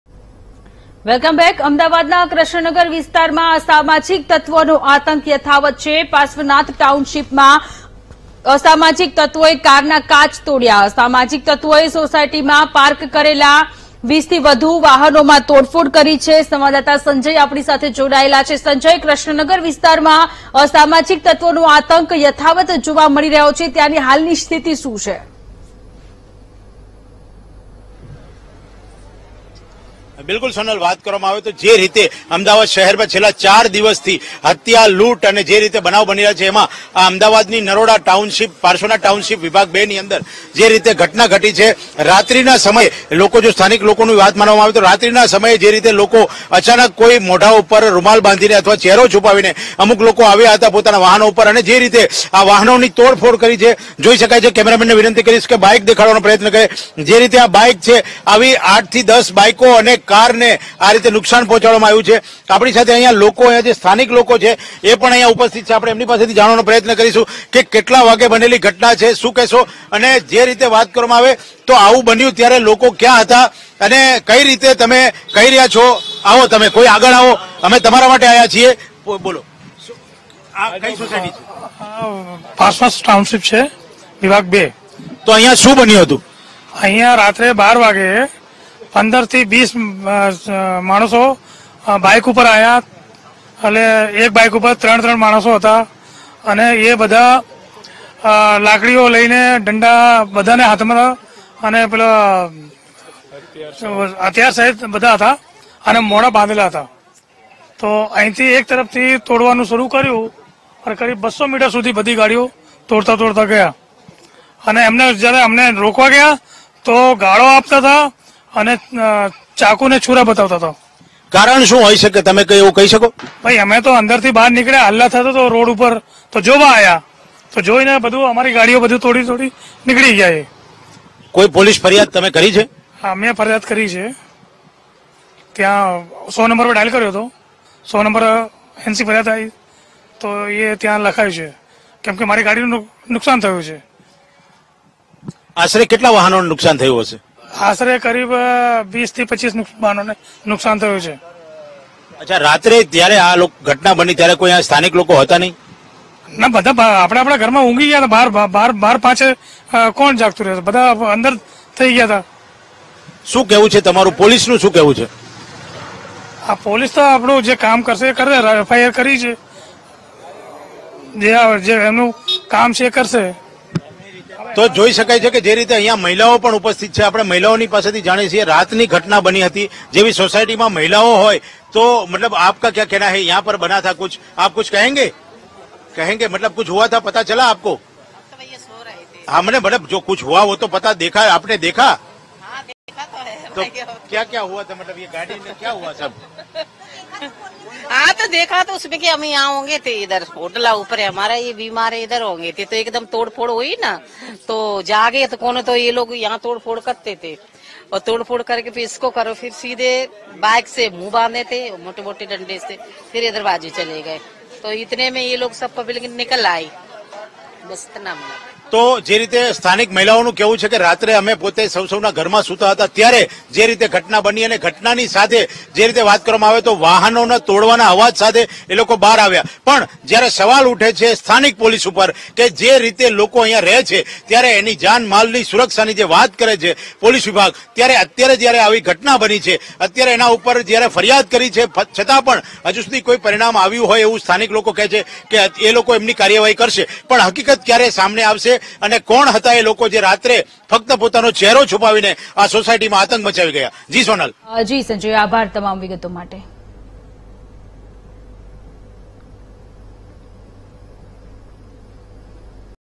Ahmedabad News: અમદાવાદના કૃષ્ણનગર વિસ્તારમાં અસામાજિક તત્વોનો આતંક | VTV GUJARATI
Ahmedabad News: પાર્શ્વનાથ ટાઉનશીપમાં અસામાજિક તત્વોએ કારના કાચ તોડ્યા, અસામાજિક તત્વોએ સોસાયટીમાં પાર્ક કરેલા 20થી વધુ વાહનમાં કરી તોડફોડ    | VTV GUJARATI